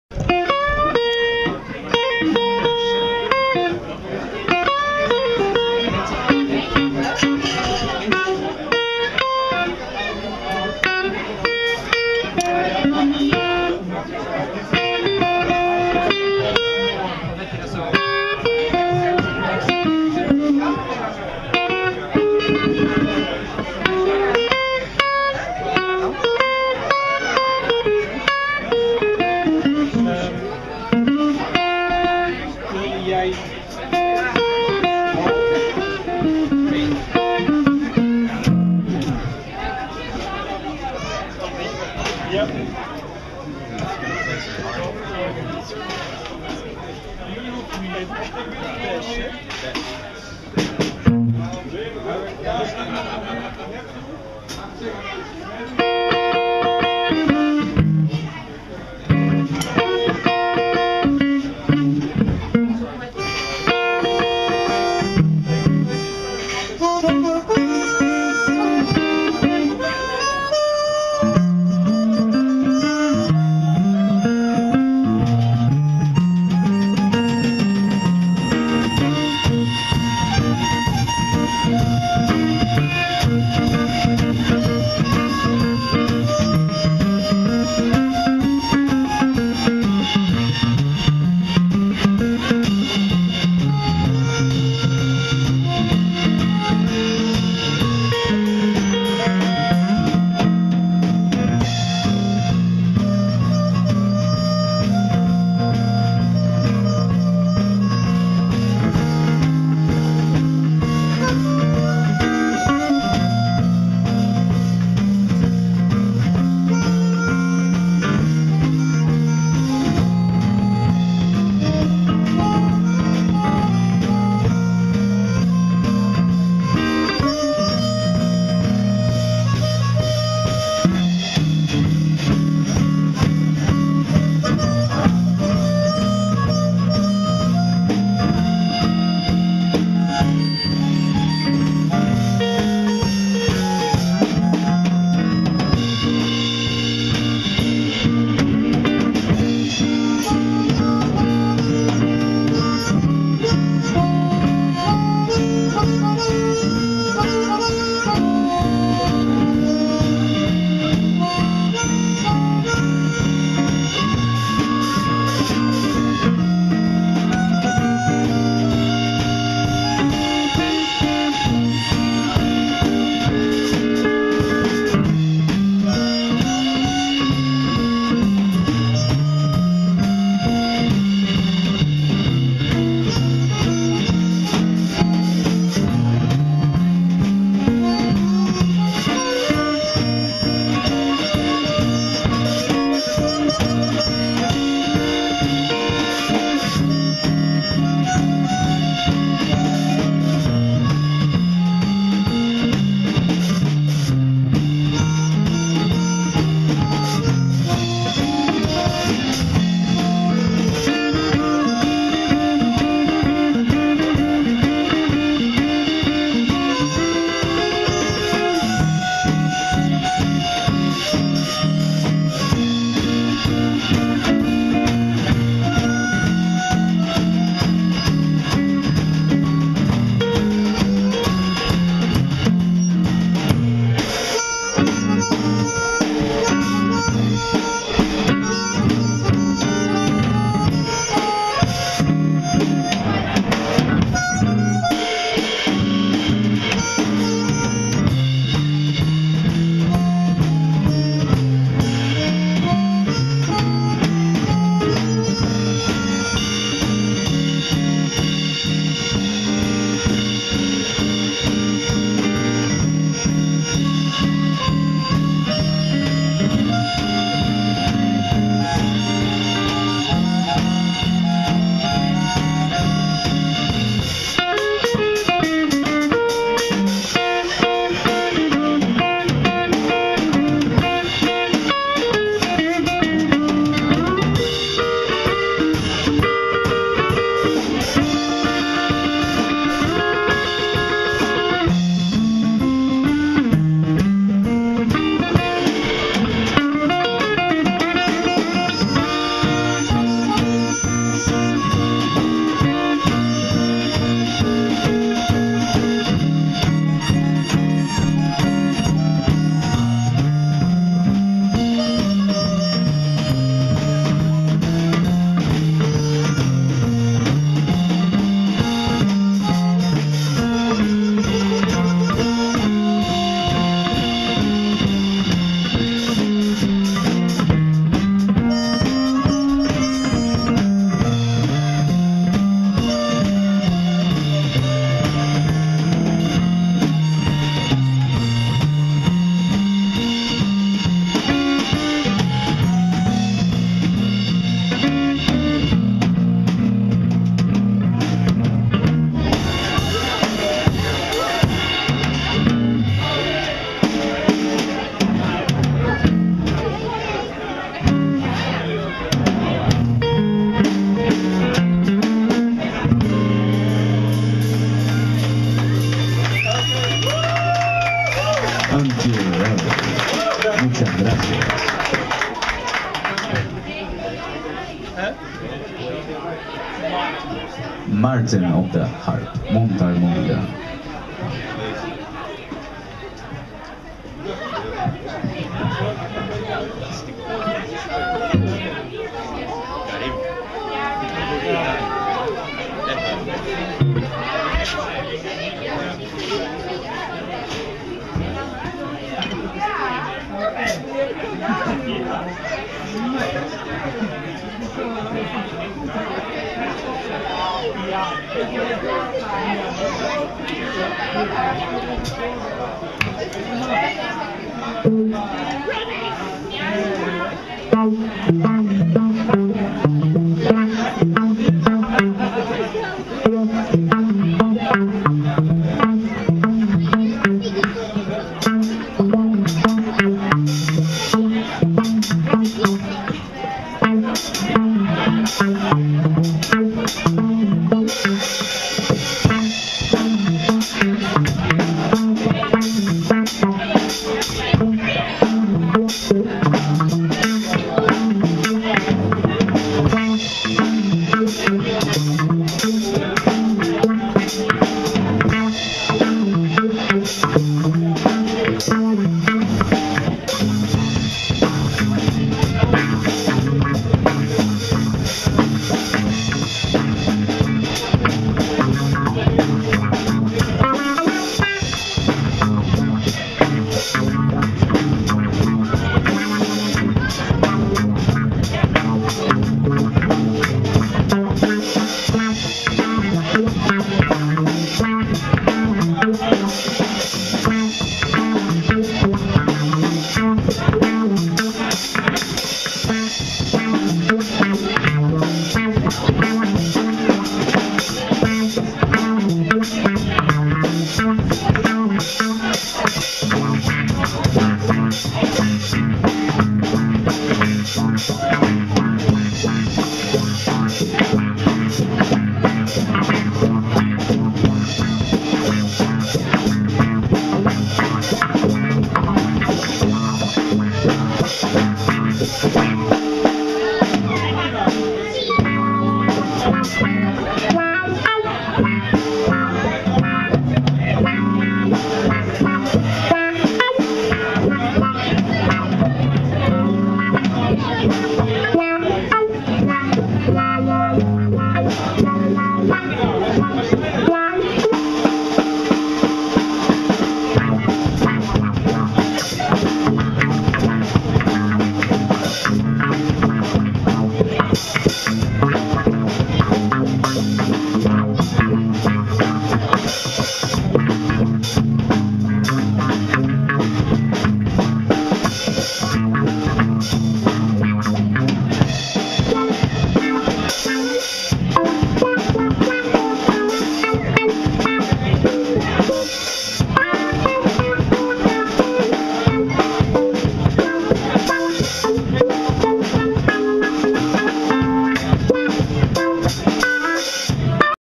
Dr Marshmallow Bluezero Jam Ijburg - 03 (Harmonica Guest)
Vicente Pino: Guitar
Steve Fly Pratt: Drums.

Blijburg January 1st 2012.

John Sinclair and his Amsterdam Blues Scholars Bliburg 2012 pt 2